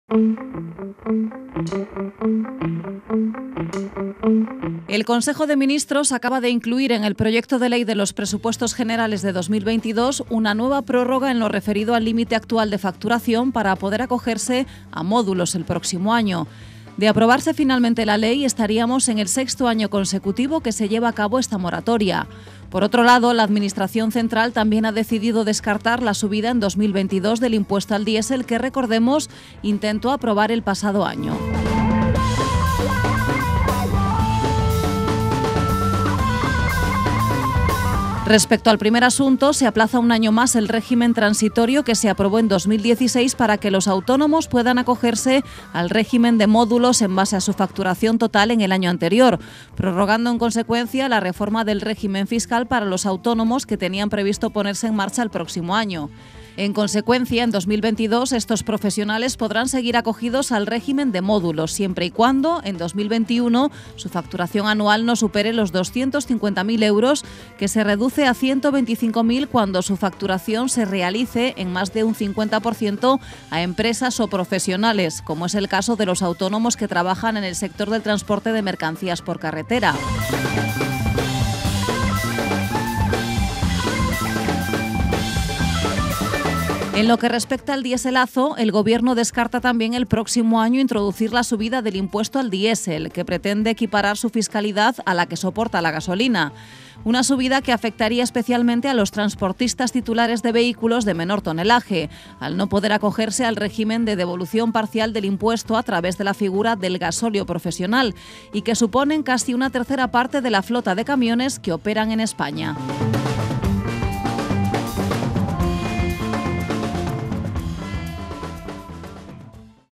CdTrucks- PRÓRROGA MÓDULOS Y DESCARTE DIESELAZO | By CdRas
El Consejo de Ministros acaba de incluir en el Proyecto de Ley de los Presupuestos Generales 2022 una nueva prórroga en lo referido al límite actual de facturación para poder acogerse a módulos el próximo año. De aprobarse finalmente la Ley, estaríamos en el sexto año consecutivo que se lleva a cabo esta moratoria. Por otro lado, la administración central también ha decidido descartar la subida en 2022 del impuesto al diesel que, recordemos, intentó aprobar el pasado año.

Respecto al primer asunto, se aplaza un año más el régimen transitorio que se aprobó en 2016 para que los autónomos puedan acogerse al régimen de módulos en base a su facturación total en el año anterior, prorrogando en consecuencia la reforma del régimen fiscal para los autónomos que tenían previsto ponerse en marcha el próximo año. En consecuencia, en 2022 estos profesionales podrán seguir acogidos al régimen de módulos, siempre y cuando, en 2021 su facturación anual no supere los 250.000 euros, que se reduce a 125 mil cuando su facturación se realice en más de un 50% a empresas o profesionales, como es el caso de los autónomos que trabajan en el sector del transporte de mercancías por carretera.

En lo que respecta al dieselazo, el Gobierno descarta también el próximo año introducir la subida del impuesto al diésel, que pretende equiparar su fiscalidad a la que soporta la gasolina. Una subida que afectaría especialmente a los transportistas titulares de vehículos de menor tonelaje, al no poder acogerse al régimen de devolución parcial del impuesto a través de la figura del gasóleo profesional, y que suponen casi una tercera parte de la flota de camiones que operan en España.